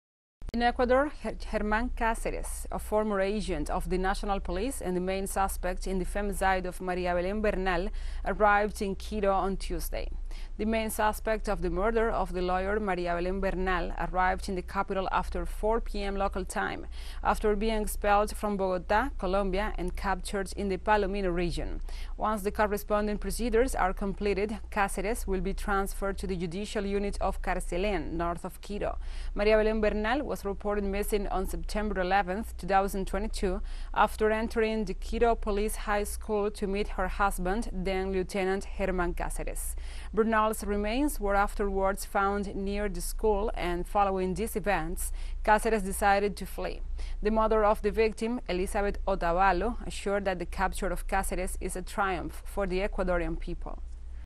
Alleged murderer of María Belén Bernal arrives in Ecuador
The former agent of the National Police of Ecuador and main suspect in the femicide of María Belén Bernal, Germán Cáceres, arrived in Quito (capital) on Tuesday afternoon. teleSUR